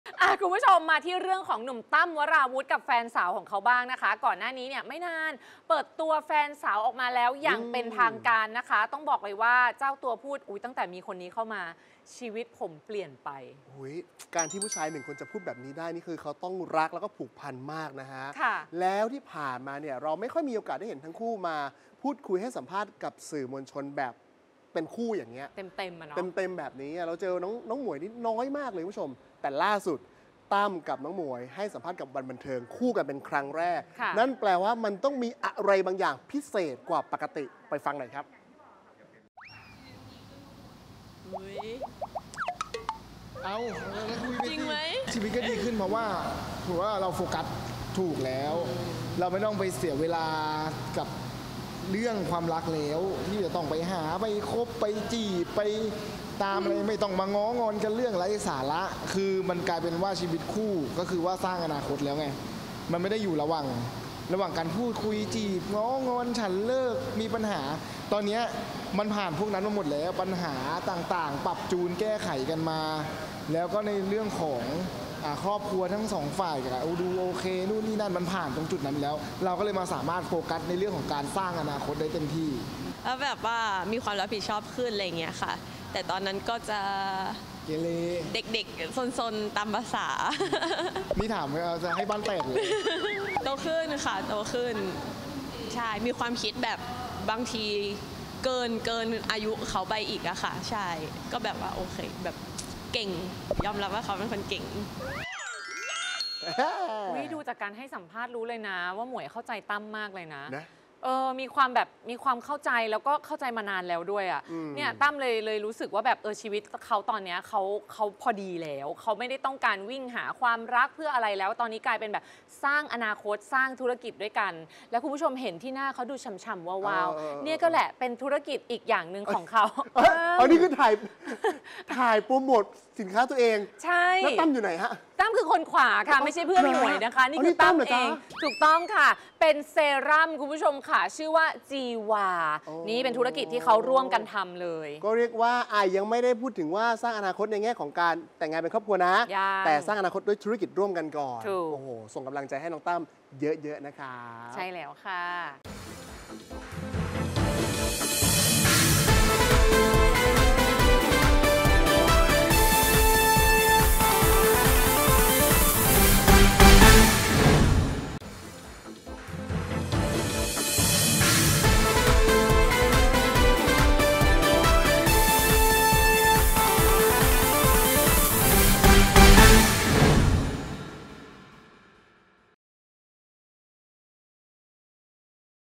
เปิดตัว “หมวย” หวานใจ “ตั้ม วราวุธ” | ข่าววันบันเทิง
หัวใจอินเลิฟ ตั้มวราวุธ เปิดตัวสาว “หมวย” หวานใจนอกวงการ ยกเป็นที่สุดของหัวใจ